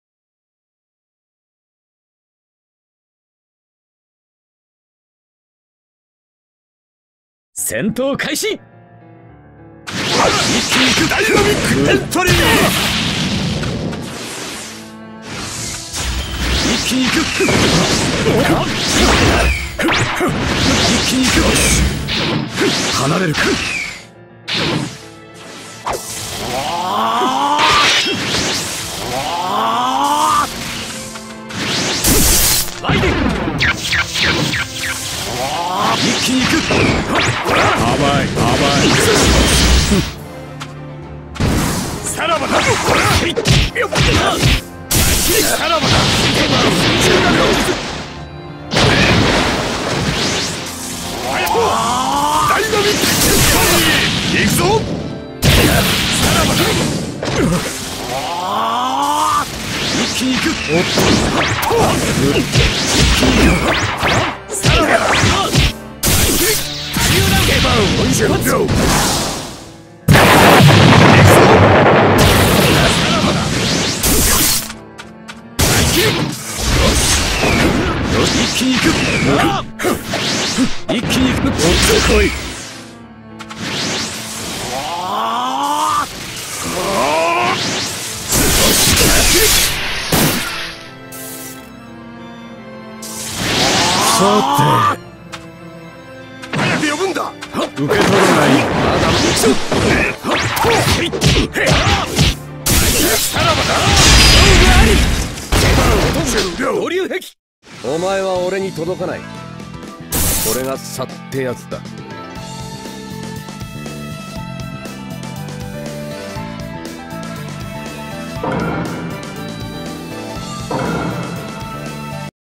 Kakashi (Gai - Hachibi) VS Tobi
Hachibi support Test

Hachibi is practically over, it remains to be tested awakening and support. but I see later for the support